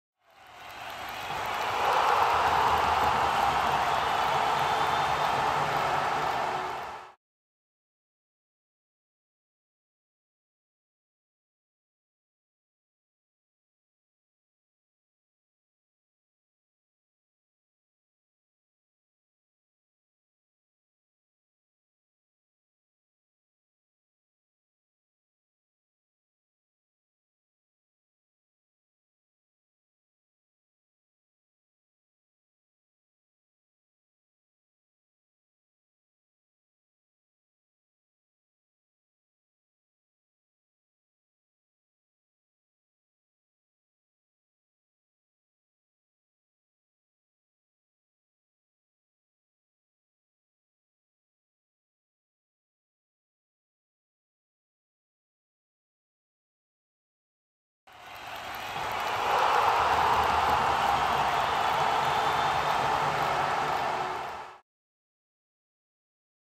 Pitch: Repeated Sprint Test
This test shows how to set up and conduct a repeated sprint test (max of 6 sprints) using either a stop watch or timing gates.